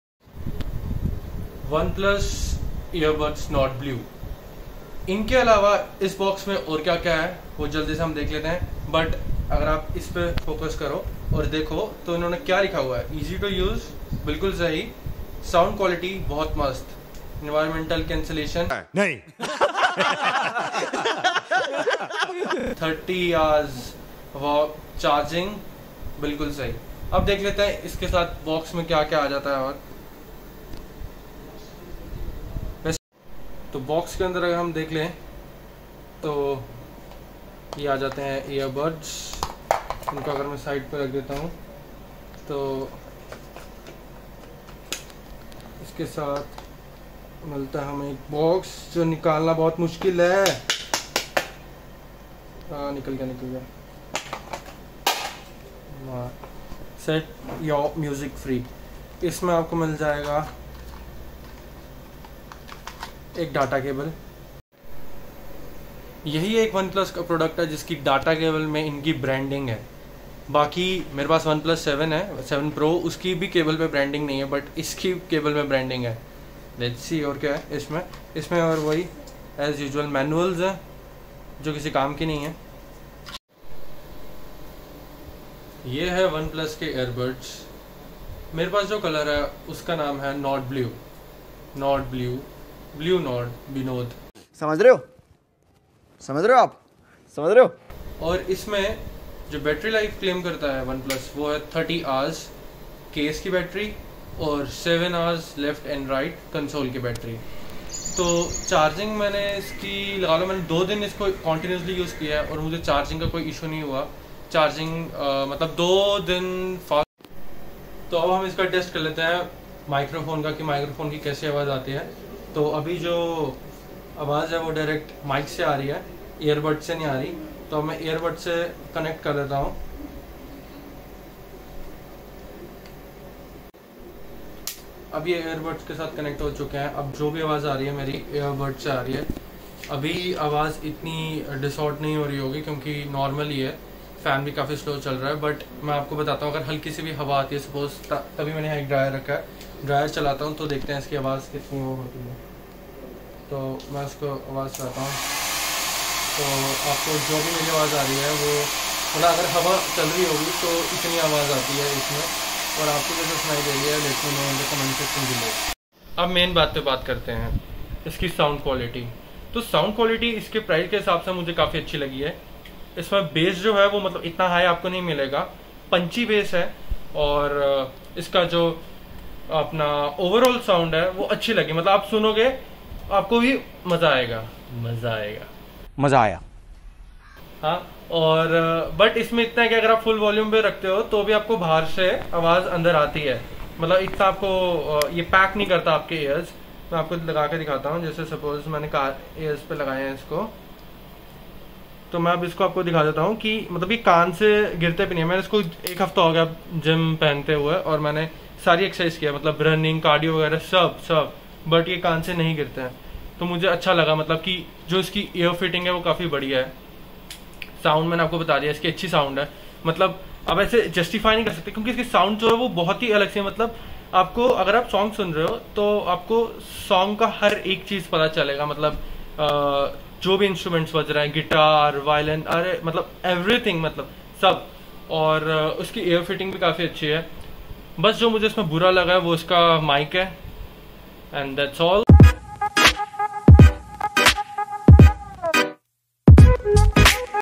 One plus earbuds | Full review | Ear Fitting | Microphone | Battery | honest review
The simple, stylish OnePlus Buds earphones offer very good wire-free sound quality for significantly less money than Apple's AirPods.